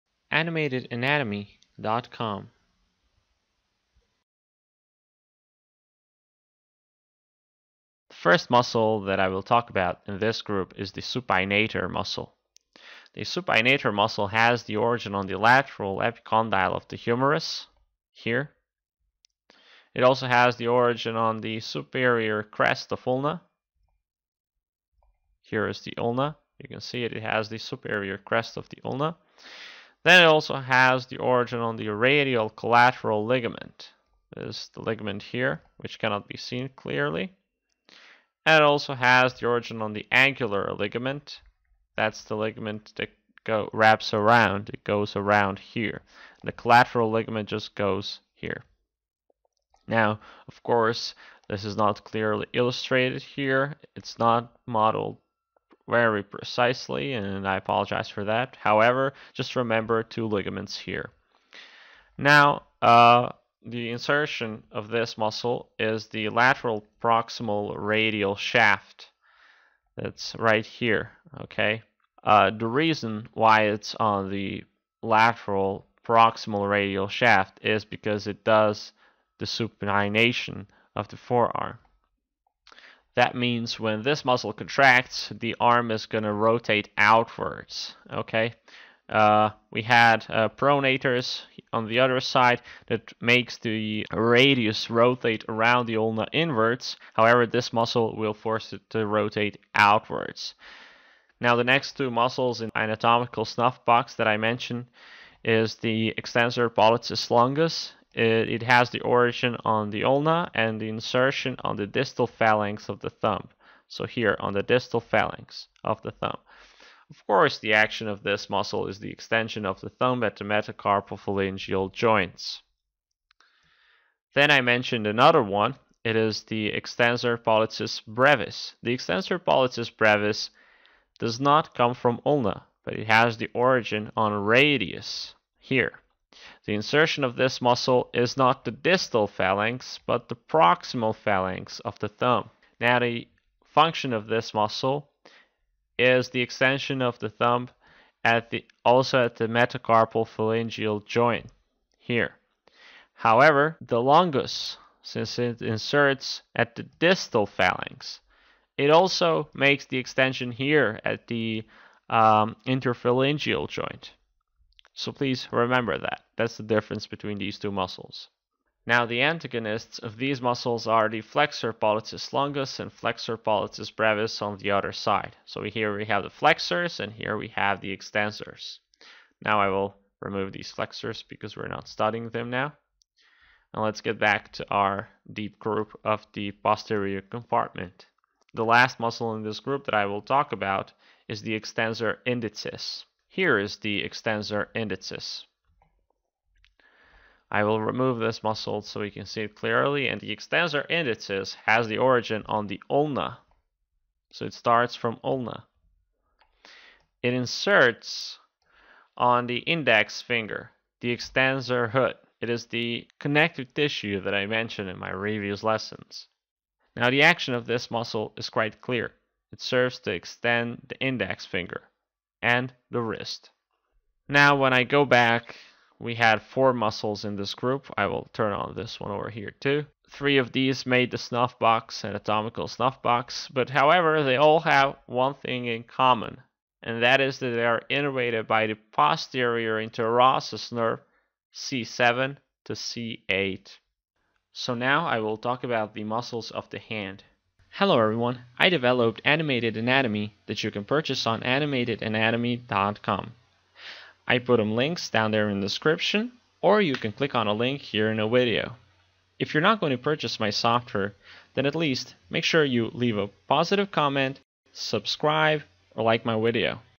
Forearm Muscles Anatomy - Posterior Compartment (Extensors) Part 2
Today I talk about  the anatomy of the forearm muscles and more precisely about the deep extensors of the posterior compartment.
Deep extensors are here: abductor pollicis longus, extensor pollicis brevis, extensor pollicis longus, supinator,. These deep muscles of the forearm have the origin on the radius and ulna and I also explain the anatomy of Ulna and Radius in my software.